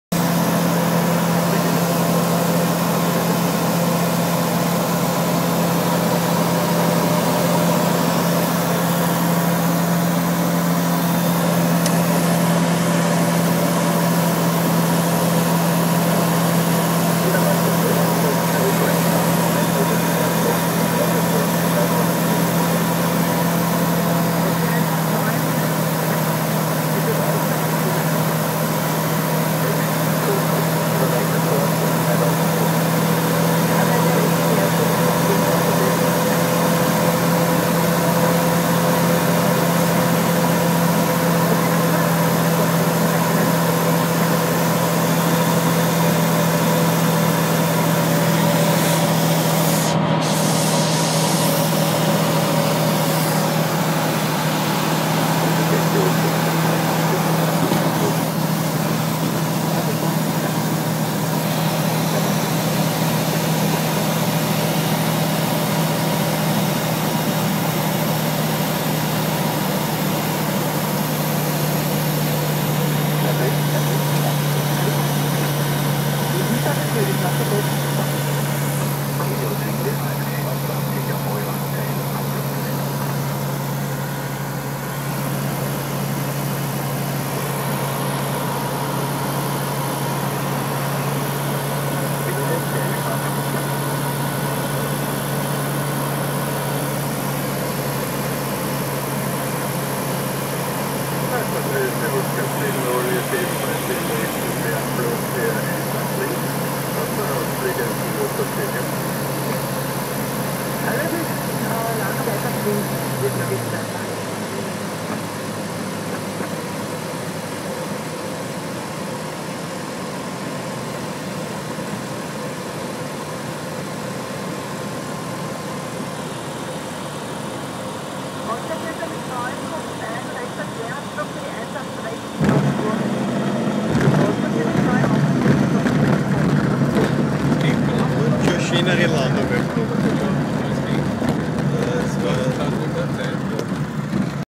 Landeanflug LOXN über Hölles
Rechter Queranflug Piste 18 L mit der Superdimona HK 36 TTC. Pilot in command Günther.